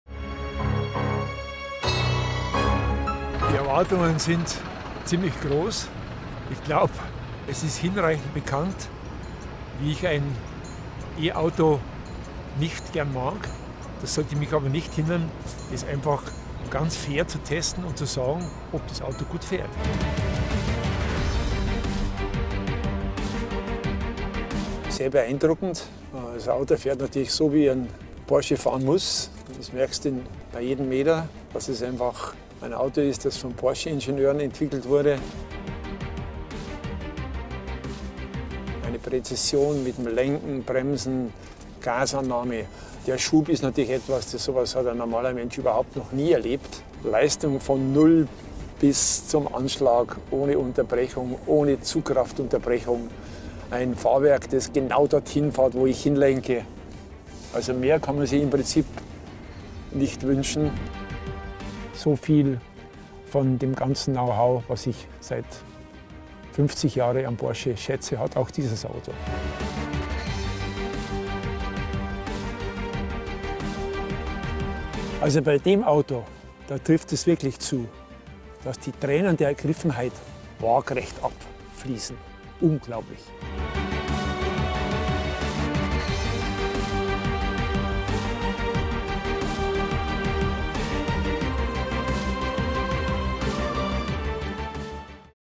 Porsche Taycan (2019) Walter Röhrl testet den Elektro-Porsche
Walter Röhrl zählt zu den besten Rennfahrern aller Zeiten und ist dazu ein bekennender Petrol-Head. Niemand würde sich wundern, wenn in seinen Adern tatsächlich Benzin statt Blut fließen würde. Jetzt ist die Rallye-Legende den neuen E-Porsche Taycan gefahren. Seine Reaktion gibt es im Video!